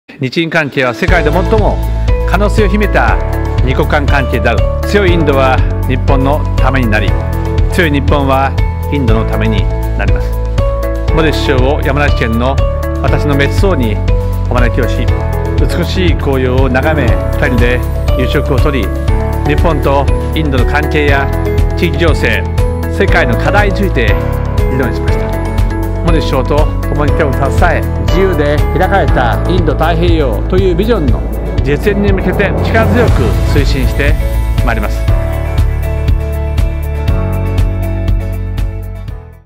日・インド首脳会談等―平成30年10月28・29日
安倍総理は、インドのモディ首相を日本にお招きし、首脳会談に臨みました。
その様子を動画にまとめましたので、是非ご覧ください。